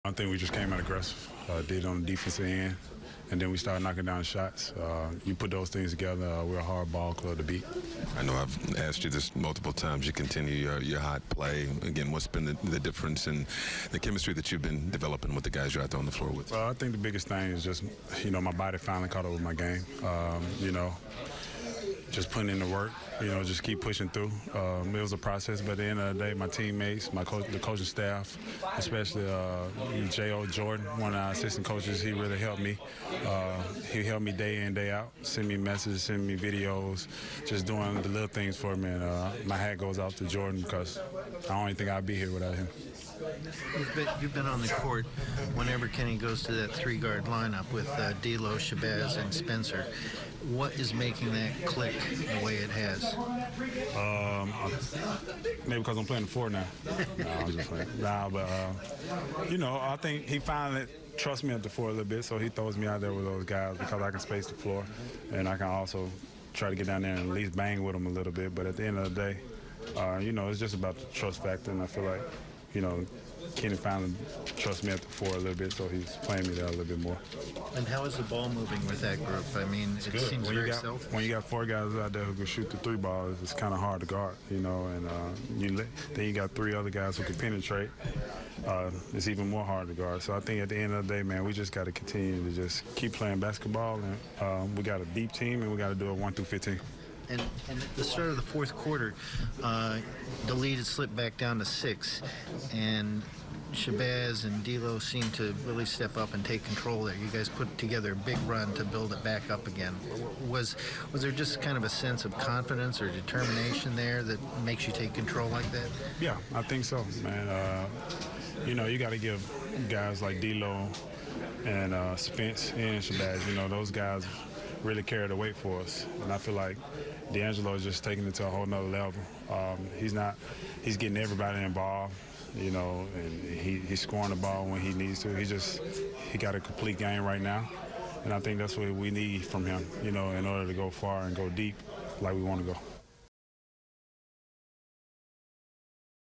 DeMarre Carroll: We're a hard ball club to beat
DeMarre Carroll after a huge Nets win over the Bulls.